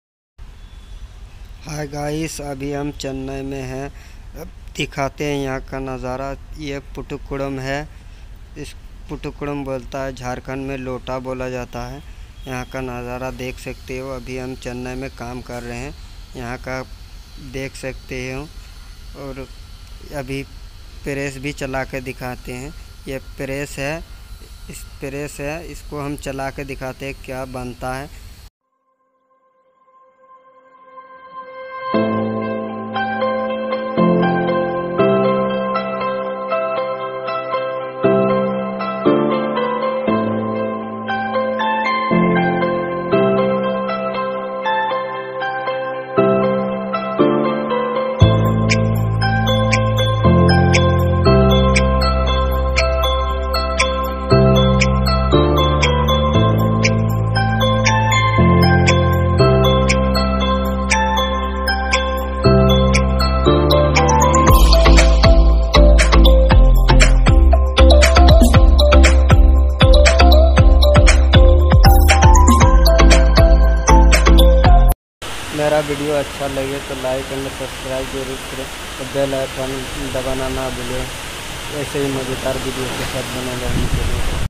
lota kaisa banta ha or plat banta ha hotbox hotpot glace tipin kari on youtube sukraliya bahi vlog
lota kaisa banta ha or plat banta ha hotbox hotpot glace tipin kari on youtube sukraliya bahi vlog Lota is made here and plate is made and hot box is made And idli plate is made gets suspended